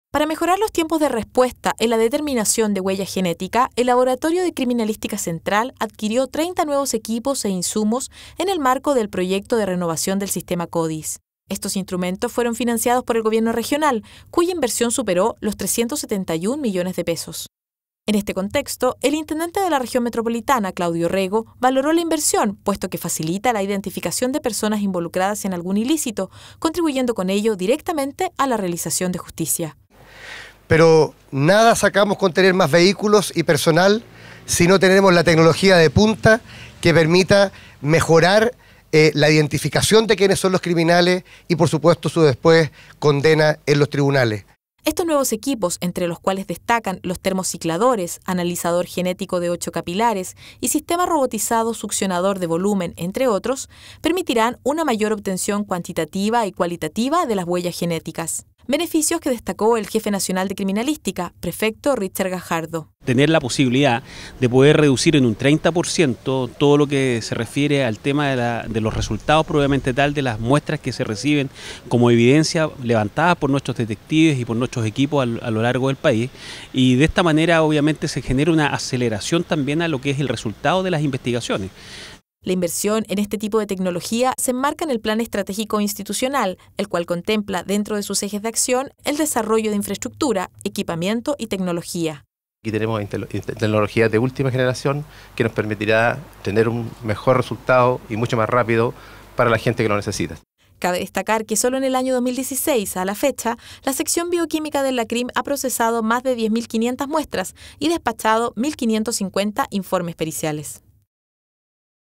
Nuevo equipamiento Codis para Lacrim Central
Gracias a la donación del Gobierno Regional, cuya inversión superó los 371 millones de pesos, la PDI adquirió tecnología de punta que permitirá mejorar los tiempos de respuesta en los resultados de las pericias, otorgando una mayor obtención cuantitativa y cualitativa de las huellas genéticas.

Algunos de estos nuevos equipos ya se encuentran operativos en el Laboratorio de Criminalística Central.